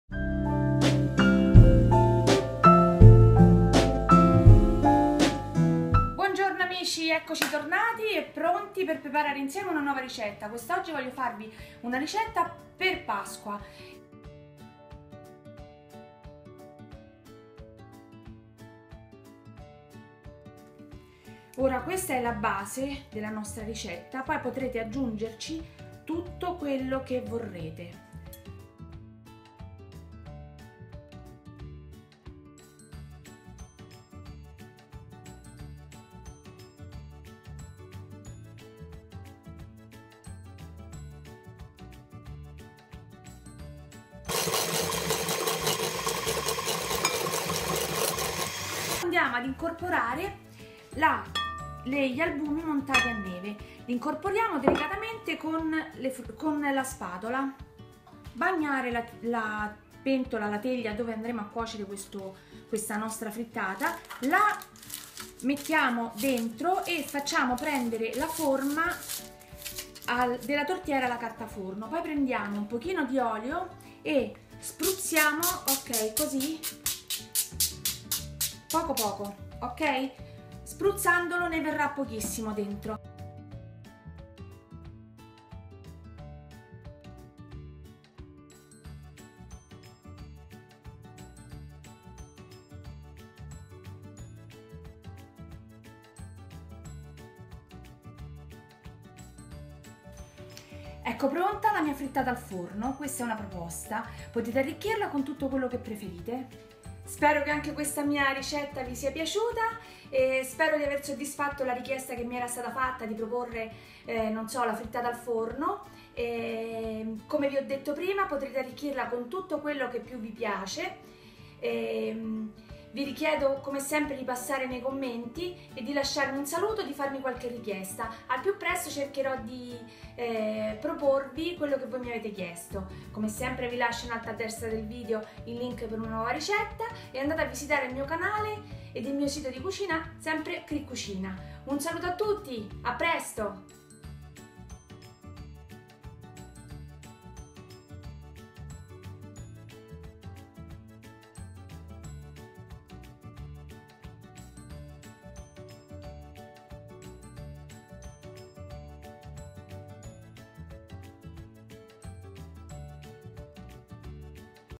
Frittata "soffiata" al forno🥚 | CriCucina
FRITTATA SOFFIATA AL FORNO
Le uova sono l'alimento che non deve mancare nelle tavole italiane il giorno di Pasqua.
Vengono cucinate in tanti modi, sode, strapazzate, frittate.
La frittata è un piatto che si puo' arricchire in tantissimi modi, può' essere messa in mezzo al pane... io oggi vi propongo la frittata al forno soffiata, cioè con gli albumi montati a neve. questo fare' si che si gonfieranno e sembrerà' una nuvola. una torta salata da decorare con tutti i salumi da voi preferiti e potrete accompagnarla alla pizza di formaggio della quale vi lascio il
Divertitevi a decorarla e mandatemi le foto sulla mia pagina fb cricucina

visitate il mio sito dove troverete tante cose interessanti.